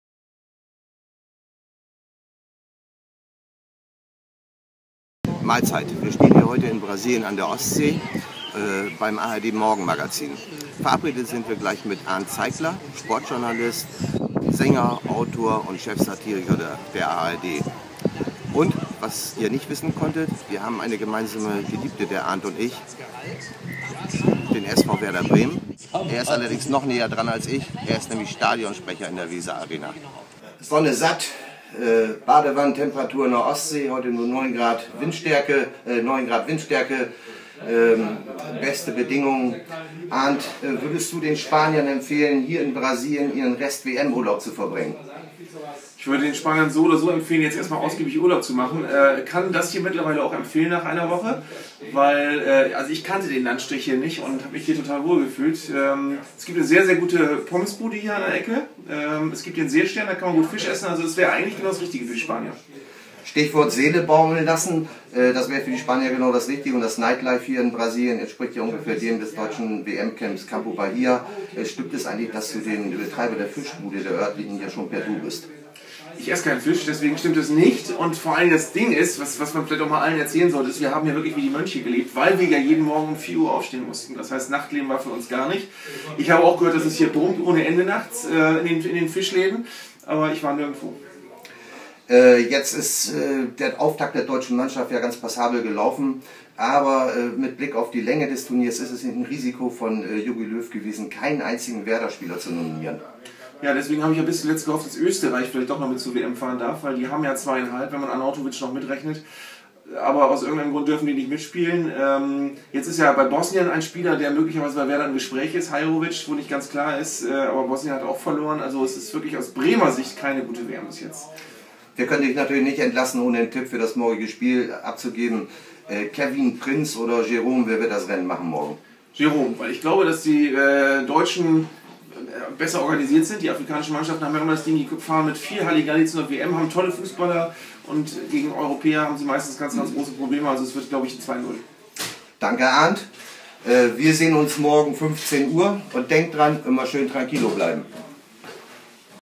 OPAcabana: Das WM-Tagebuch (Tag 9)
Andreas "Opa" Geidel hat sich auf den langen Weg nach Brasilien gemacht, um dort mal mit einem echten Experten zu sprechen. Arnd Zeigler erklärt von Werder-Fan zu Werder-Fan, wie schön es bei uns ist und warum kein Werder-Profi in der Natze spielt.